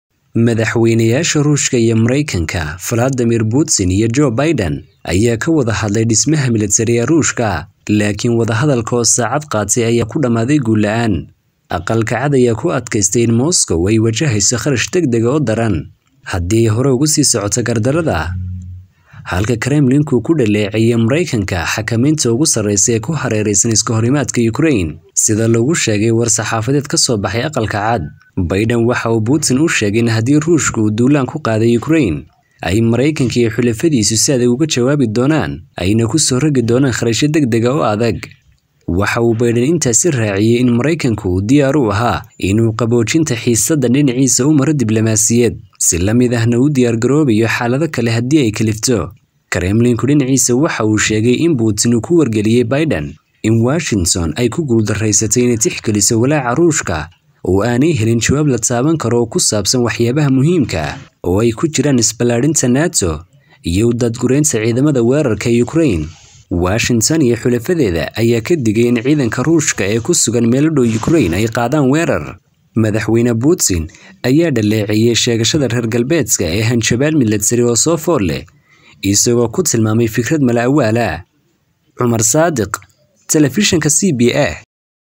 Madaxweynayaasha Ruushka & Maraykanka oo wadahadal ay ka yeesheen xiisadda Ukrain...
Madaxweynayaasha Ruushka iyo Maraykanka oo wadahadal ay ka yeesheen xiisadda Ukrain uu guul la'aan ku dhammaaday.